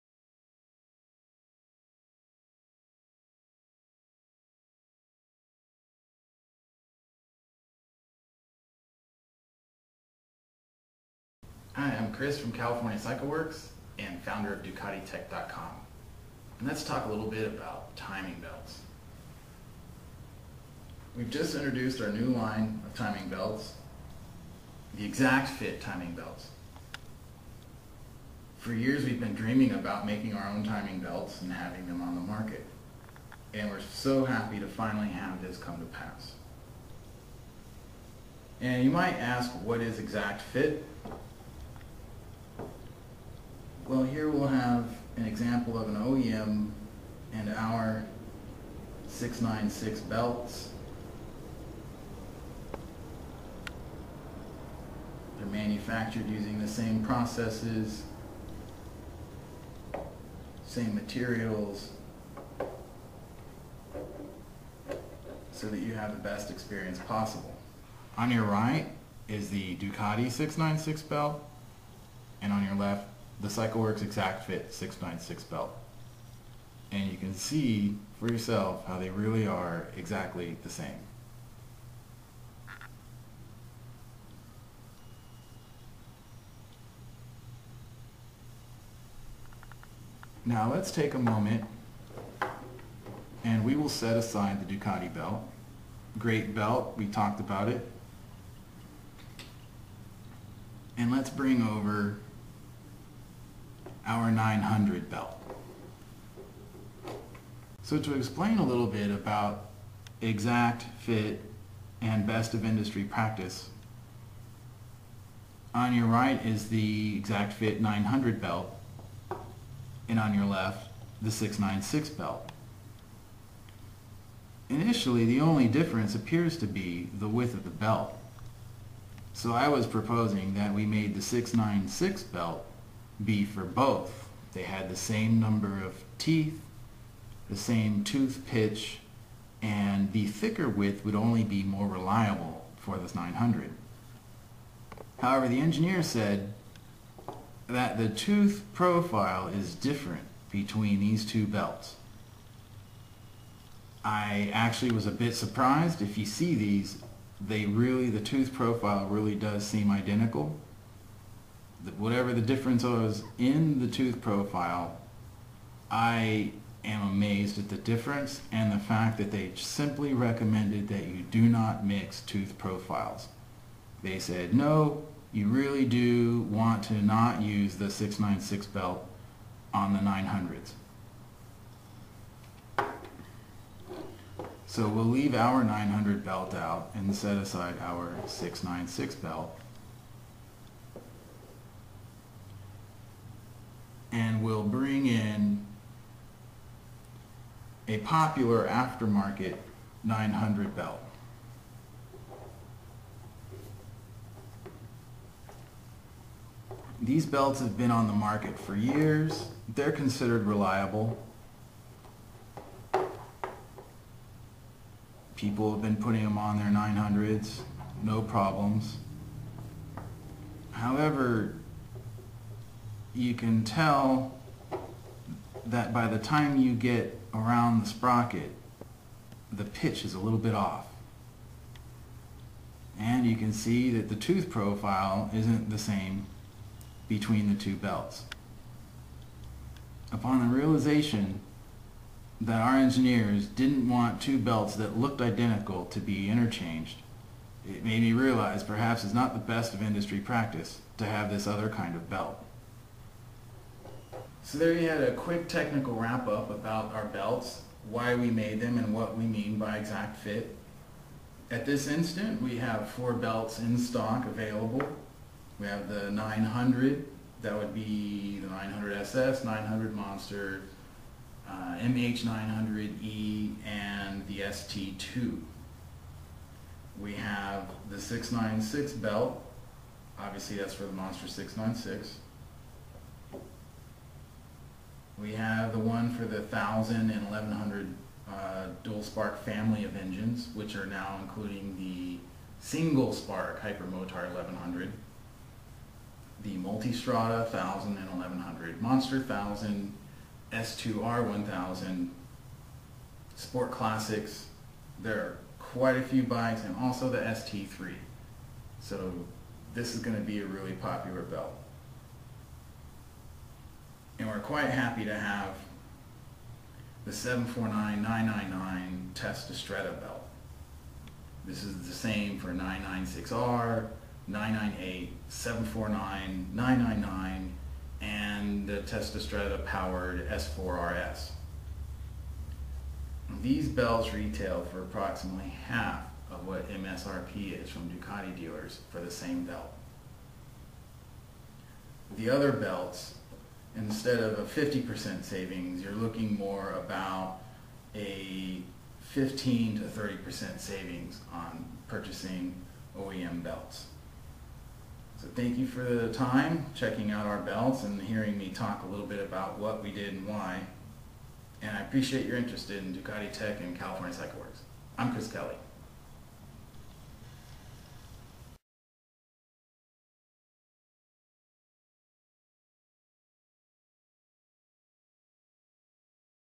Ducatitech.com: ExactFit Timing Belts
A short technical comparison of timing belts for Ducati Motorcycles by Chris Kelley. This video walks you through an analysis of California Cycleworks ExactFit, Ducati OEM and other aftermarket timing belts.